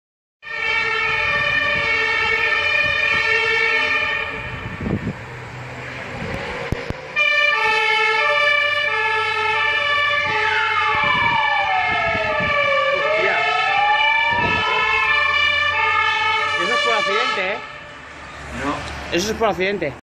(1/12/2014) Bomberos de Bilbao en camino a un accidente.
S6, UMJ-2 y de Bomberos Bilbao del parque de Miribilla a su paso por Txurdinaga para accidente en Zabalbide (Carretera de Artxanda). Furgoneta volcada.